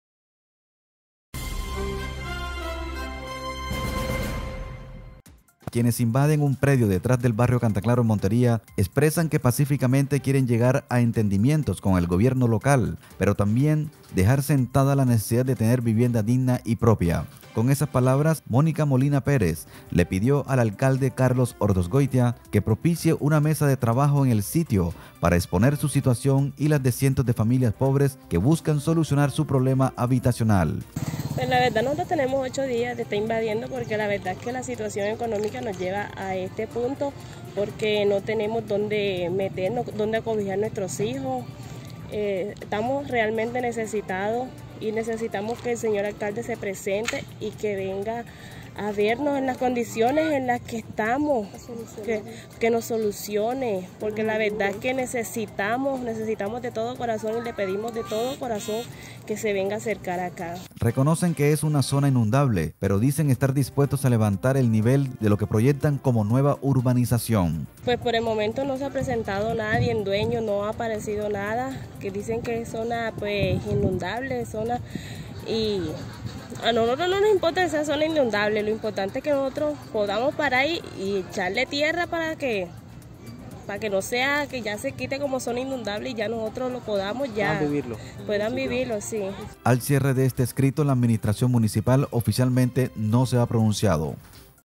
Diálogo con el alcalde de Montería, piden invasores en Cantaclaro
Quienes invaden un predio detrás del barrio Cantaclaro en Montería, expresan que pacíficamente quieren llegar a entendimientos con el gobierno local, pero también dejar sentada la necesidad de tener vivienda digna y propia.